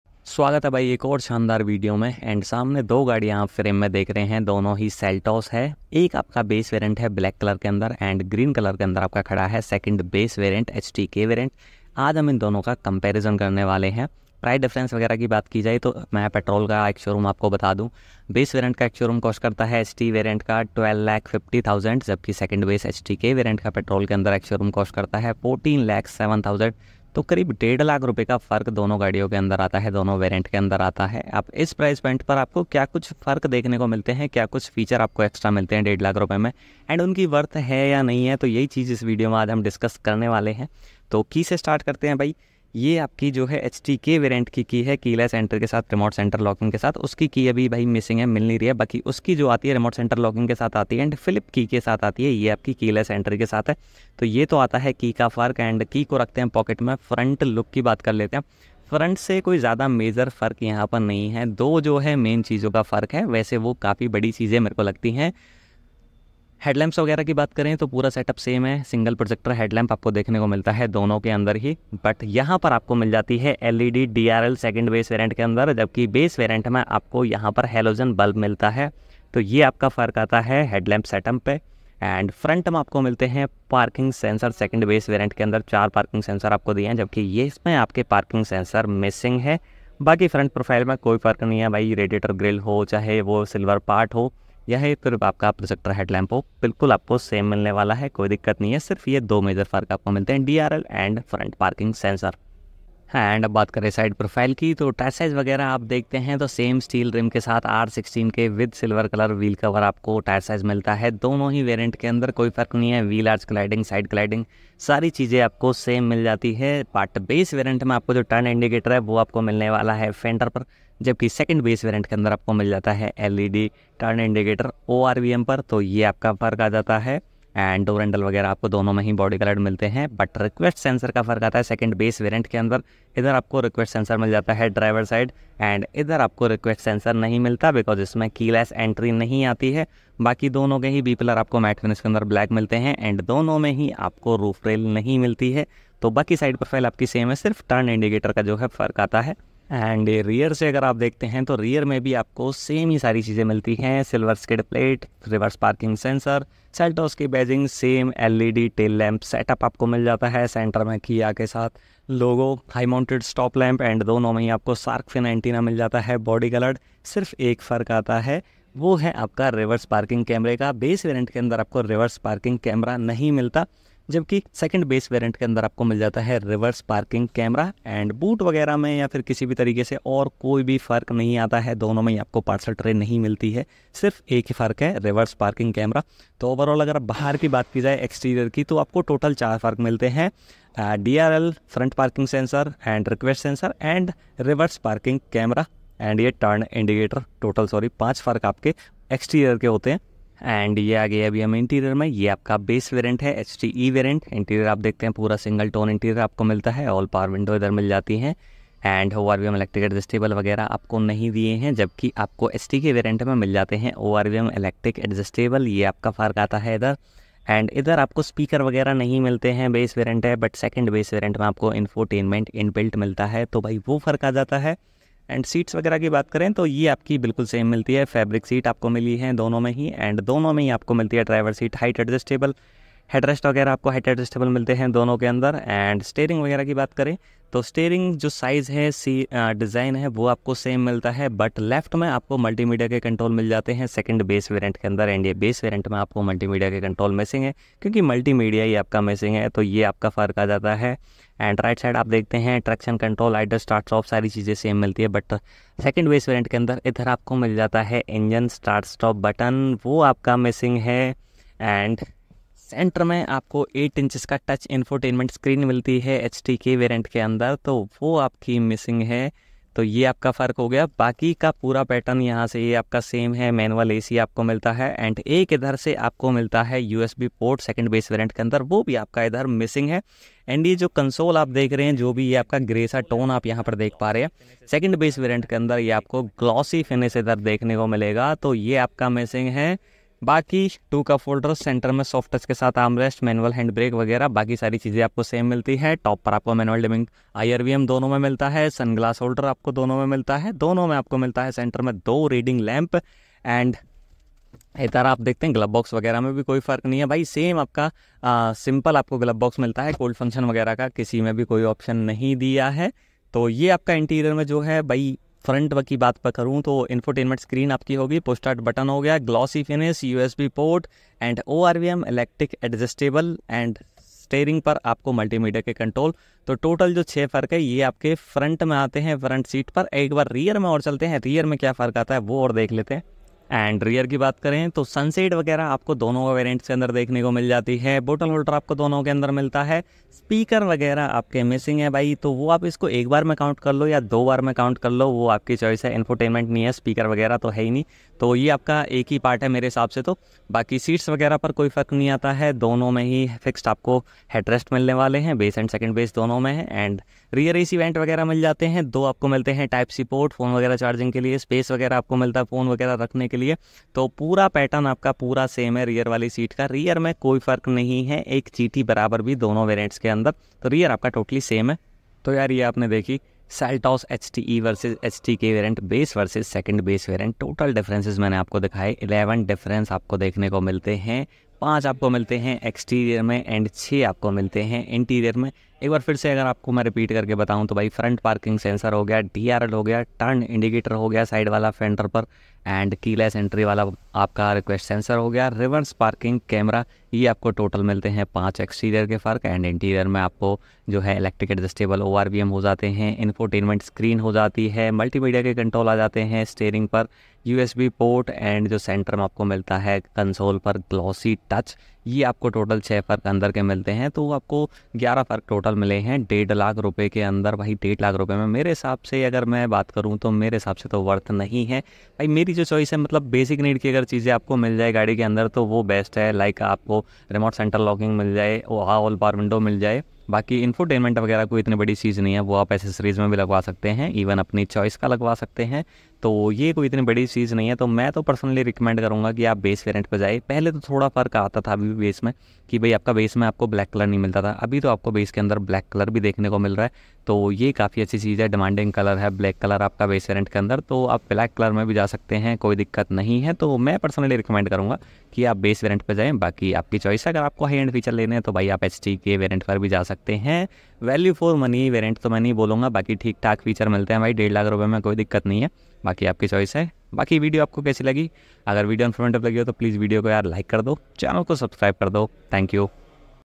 Kia seltos HTE vs HTK 2024 | Seltos Base Vs 2nd Base Variant every single details | The AutoWorld
Hello Everyone, This side Arun Nagar